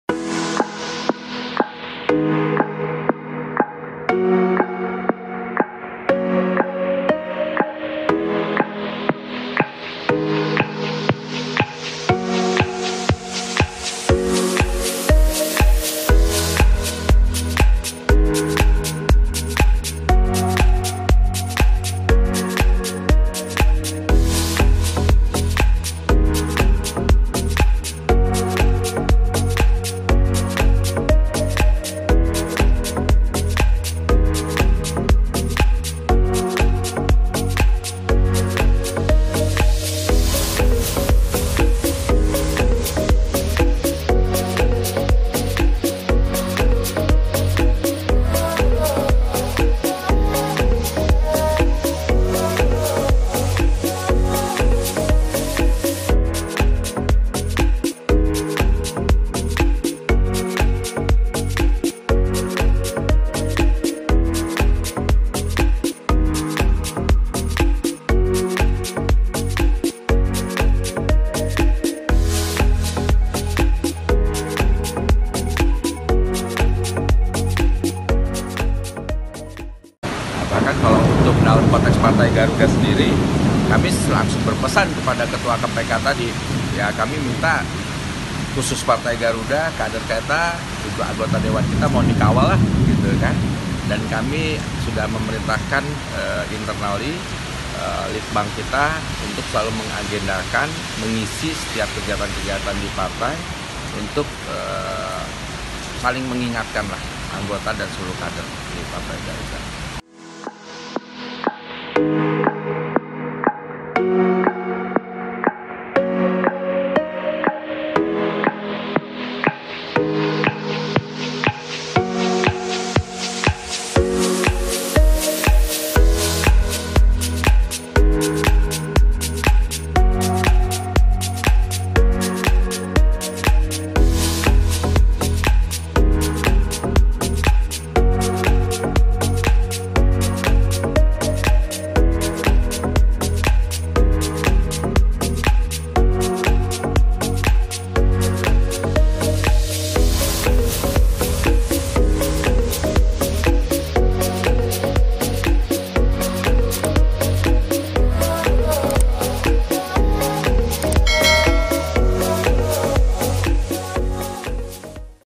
Partai Garuda Ikuti Program Pembekalan Antikorupsi dari KPK
Partai Gerakan Perubahan Indonesia (Garuda) mengikuti pembekalan antikorupsi dari KPK pada Rabu (22/6/2022) di Gedung Merah Putih KPK.

Ketua Umum Partai Garuda Ahmad Ridha Sabana meminta KPK mengawal kader Partai Garuda untuk mencegah terjadinya korupsi dan Partai Garuda juga akan membuat agenda rutin pembekalan antikorupsi.

Pembekalan antikorupsi ini merupakan salah satu rangkaian program Politik Cerdas Berintegritas (PCB) dari KPK yang diberikan kepada 20 pimpinan dan pengurus partai politik (parpol).

Video Jurnalis: Irfan Kamil, Talitha Yumnaa
Penulis: Irfan Kamil
Penulis Naskah: Talitha Yumnaa
Video Editor: Talitha Yumnaa
Produser: Khairun Alfi Syahri MJ